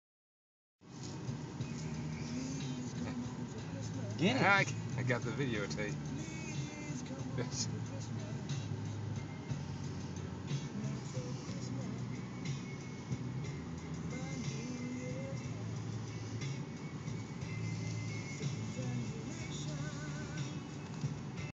Dog molesting a Goat How new animals are made
Was just driving down the highway and this is what we saw
Dog doing a Goat